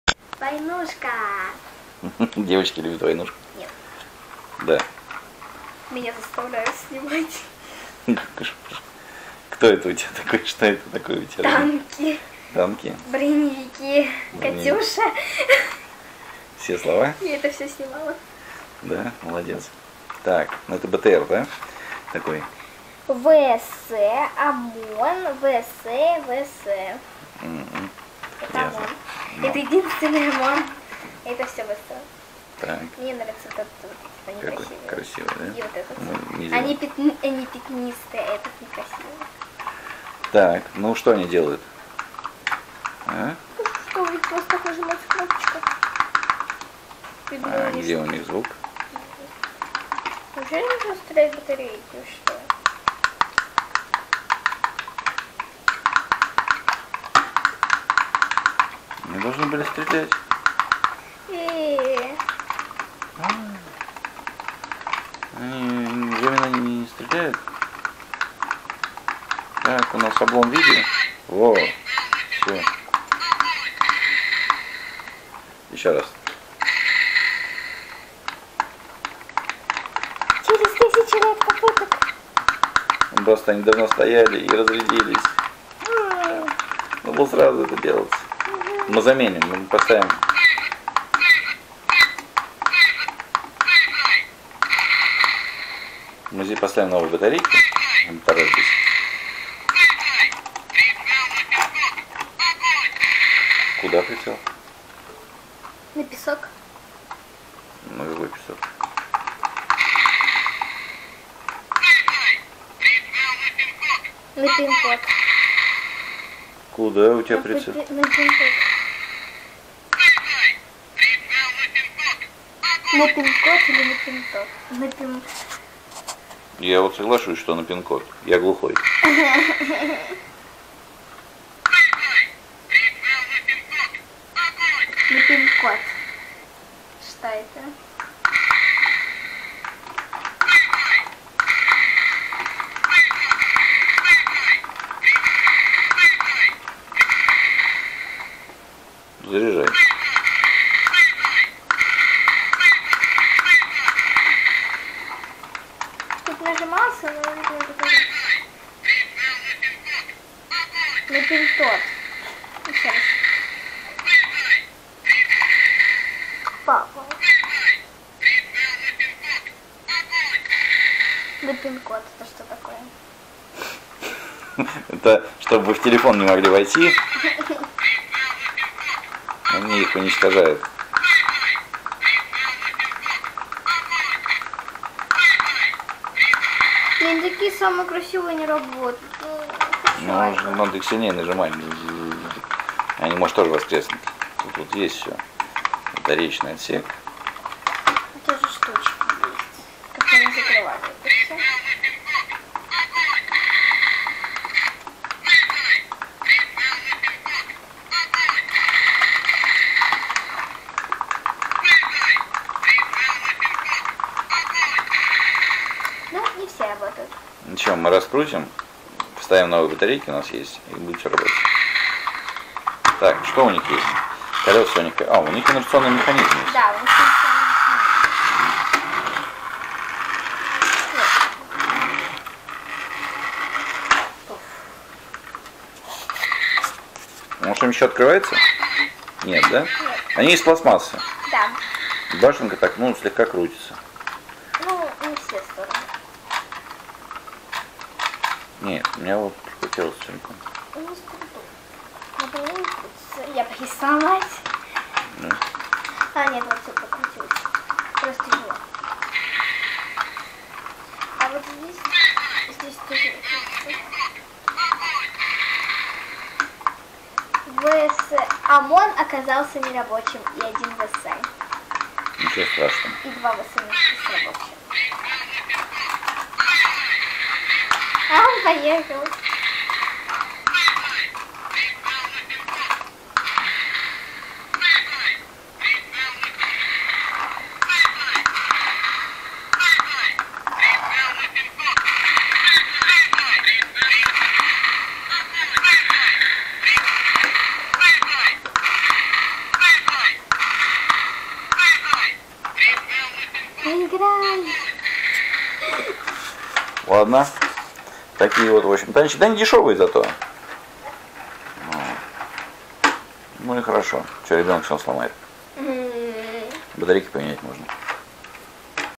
Игрушечный военный БТР
в длину 13,9 см
вращается башня, поднимается вверх-вниз пушка
световые и звуковые эффекты